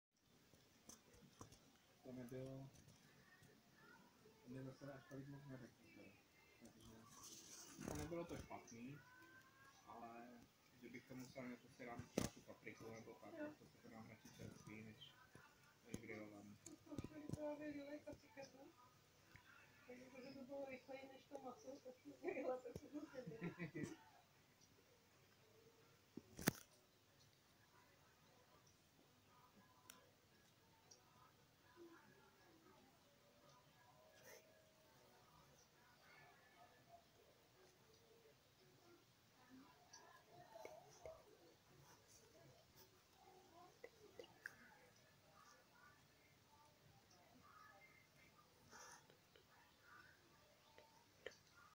Test kvality videa - Nokia 3.1 - den
Test kvality videa telefonu Nokia 3.1 za denního světla